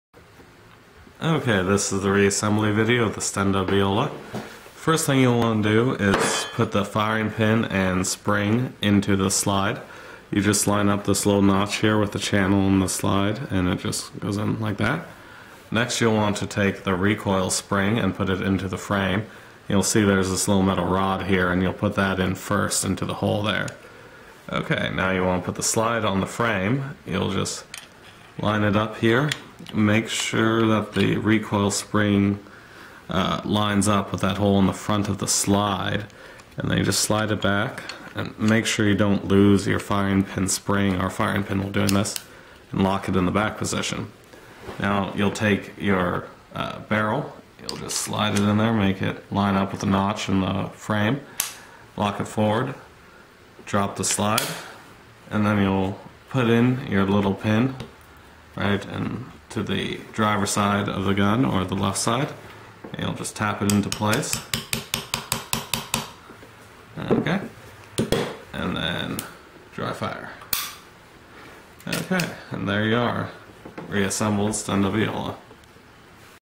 Stenda Beholla Reassembly
Reassembly of a Stenda Beholla pistol.